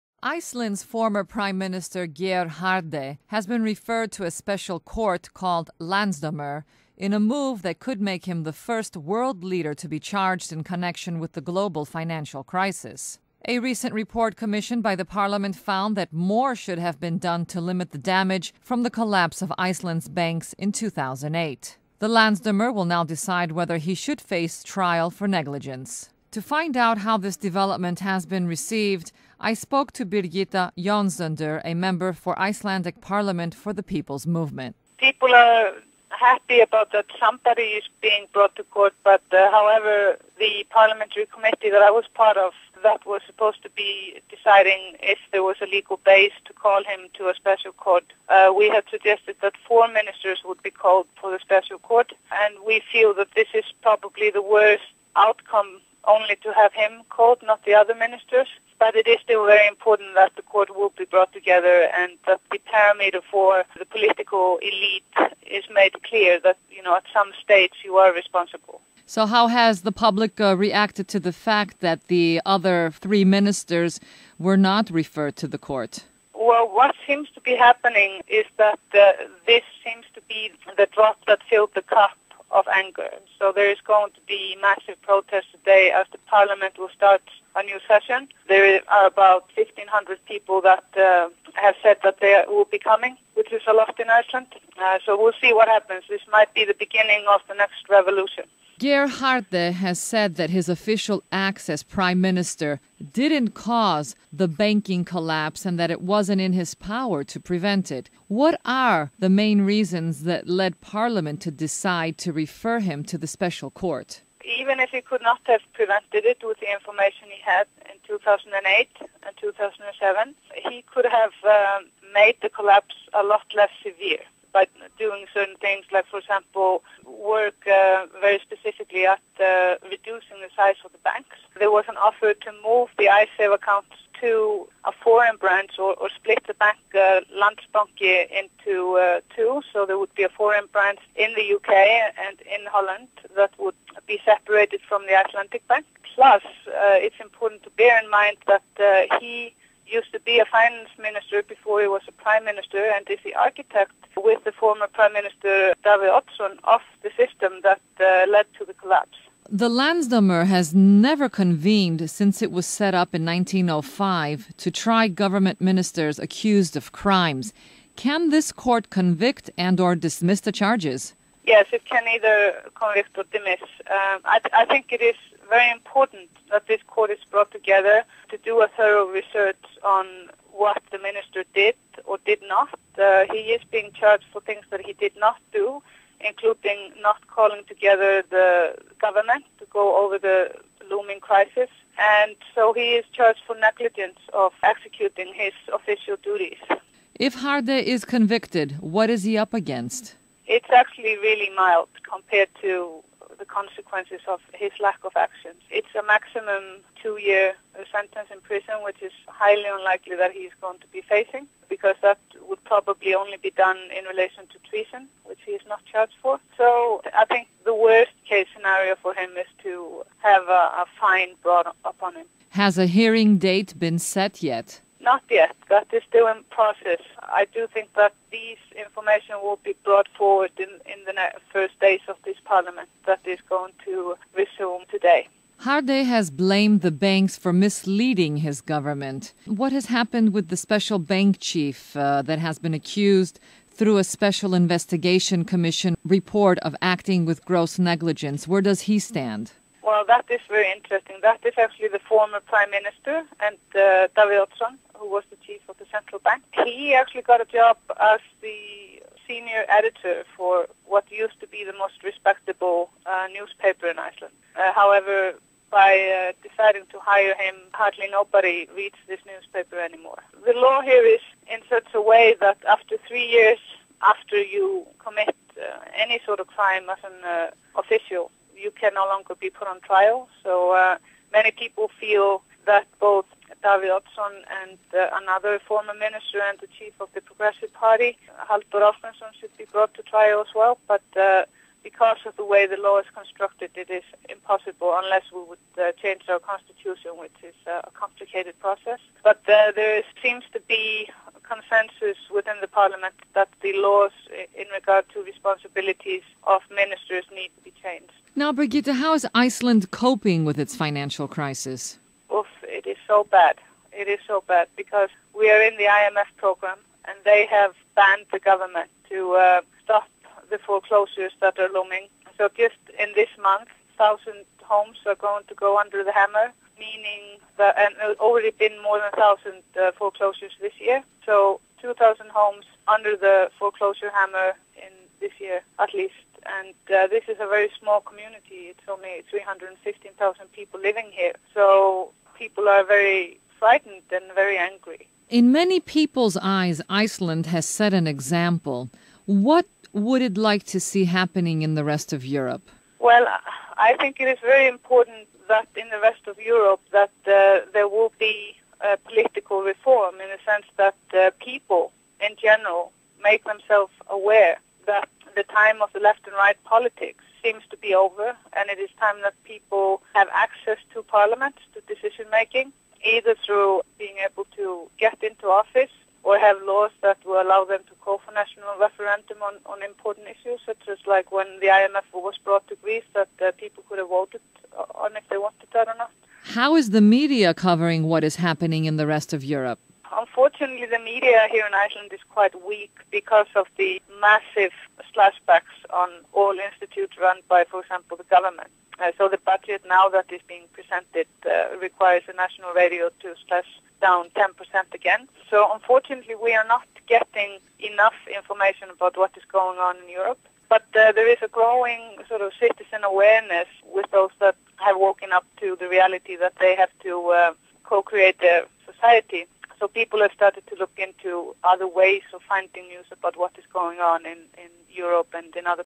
ICELAND FIGHTS BACK "people should have access to Parliament and decision making"Birgitta Jonsdottir
Iceland's former Prime Minister Geir Haarde has been referred to a special court called the Landsdómur in a move that could make him the first world leader to be charged in connection with the global financial crisis. A recent report commissioned by the parliament found more should have been done to limit the damage from the collapse of Iceland's banks in 2008. To find out how this development has been received, Helen Skopis spoke with Birgitta Jonsdottir, a member for Icelandic parliament for the People's Movement,  asking for insight on the following questions:

How has the public reacted to the fact that three additional ministers who were being investigated were not referred to the court?

Geir Haarde has said that his official acts as Prime Minister didn't cause the banking collapse and it was not in his power to prevent it. What are the main reasons which lead the parliament to decide to refer him to the special court?

The Landsdómur has never convened since it was set up in 1905 to try government ministers accused of crimes. Can this court convict and our dismiss the charges?

Has a hearing date been set yet?

Haarde has blamed the banks for misleading his government. What has happened with the special bank chief who has accused thru a special investigation report of acting with gross negligence? Where does he stand?

How is Iceland coping with it's financial crisis and the presence of the IMF?
In many people's eyes the citizens of Iceland have set an example for the world by standing up to the banking system... what would Iceland like to see happening in the rest of Europe?

Birgitta Jonsdottir answers Helen Skopis in detail describing the people's reaction to Iceland's  historic vote, the rage of many citizens concerning their dire economic conditions, the increase in home foreclosures and Jonsdottir's own hope that there will be political reform  in the rest of Europe. She hopes the people of Europe become aware that "...the age of "left and Right" politics is over and it is time people have access to Parliament and to decision making either thru being able to get into office  or thru laws which will allow them to call for national referendums on important issues such as when the IMF was brought into Greece. "